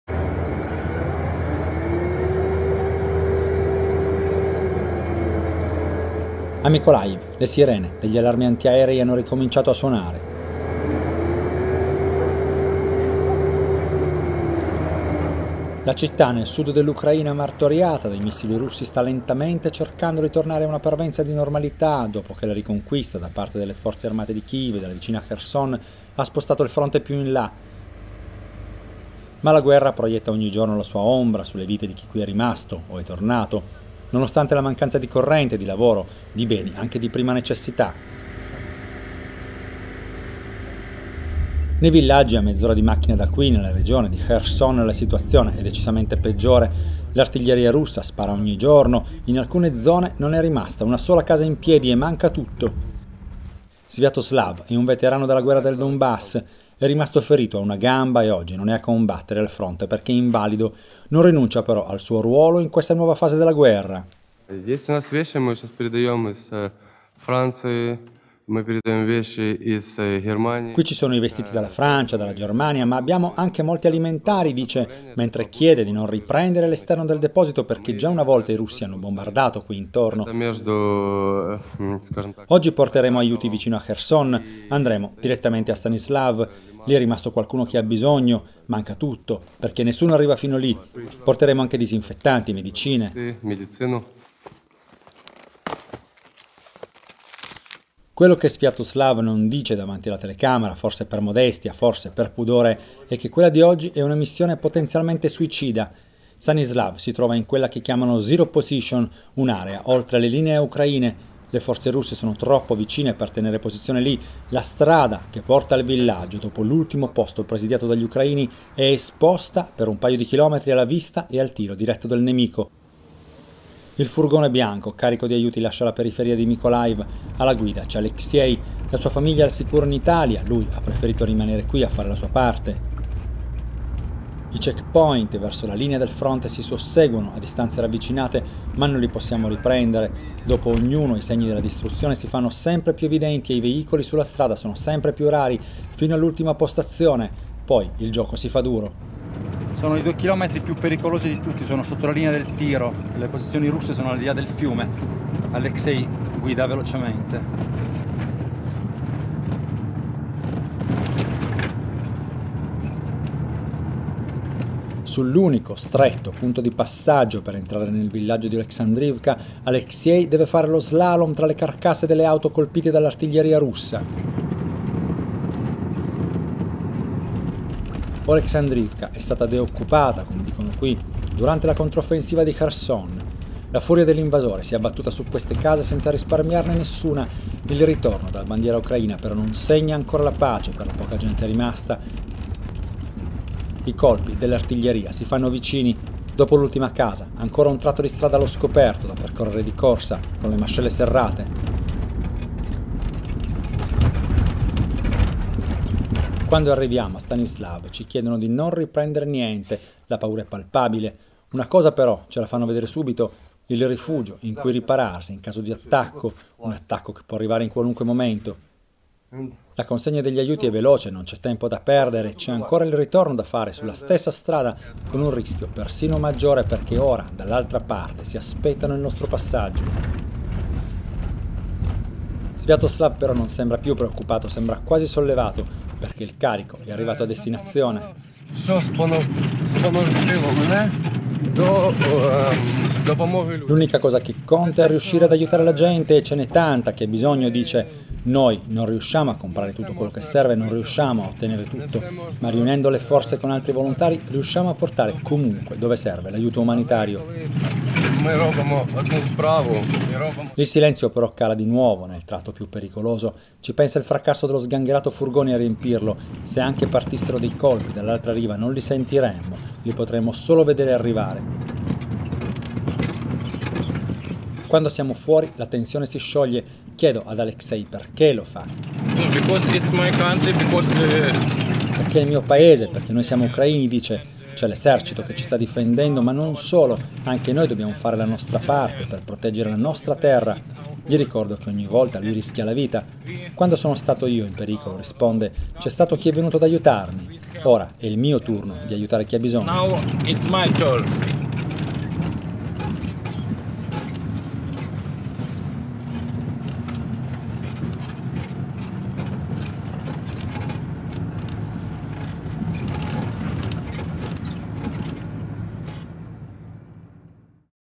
Ucraina: i volontari kamikaze
Mykolaiv è la città che ha fermato l’avanzata russa verso Odesa. Colpita ripetutamente dai missili russi, bombardata, ferita, ora trattiene il fiato. Respinti i russi oltre oltre Kherson, Mykolaiv è il primo baluardo sicuro sul fronte meridionale. Fuori dal tiro dell’artiglieria - ma pur sempre a portata dei missili russi - è il posto dove i militari in rotazione dal fronte passano le loro brevi licenze prima di tornare in trincea. Ma è anche la base delle tante organizzazioni di volontari che portano aiuti alle popolazioni sul fronte: basta una mezz'ora di macchina per arrivarci e c'è chi rischia la vita per aiutare chi letteralmente vive sotto le bombe.

Un videoreportage di Danilo Elia